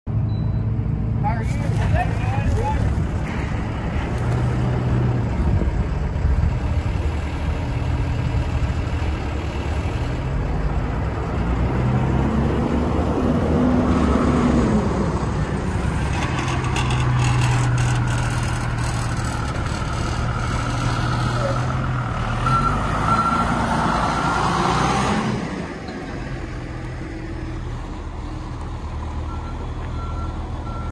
2004 BLUEBIRD ALL AMERICAN For Sale
Cummins 24 valve, Allision Automatic, 84 passenger, All american package, runs and drives, floor has patch, rear engine, has oil leak underneath up front.